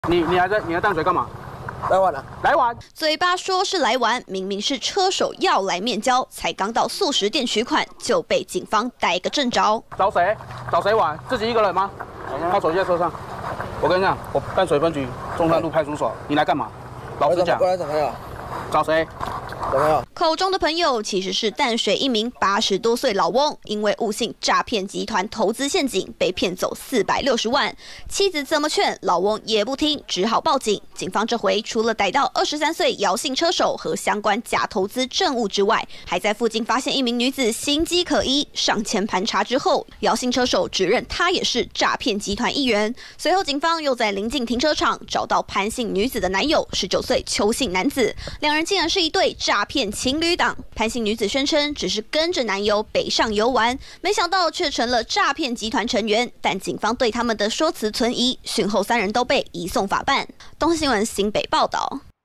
8旬翁遭詐460萬！ 警逮車手和19歲詐騙情侶檔@newsebc
新北淡水一名80歲老翁因為誤信詐騙集團投資陷阱，被騙走460萬元，妻子緊急之下求助警方。警方假借老翁之名和車手再約面交，除了現場逮捕姚姓車手，還在附近逮到同夥的19歲詐騙情侶檔。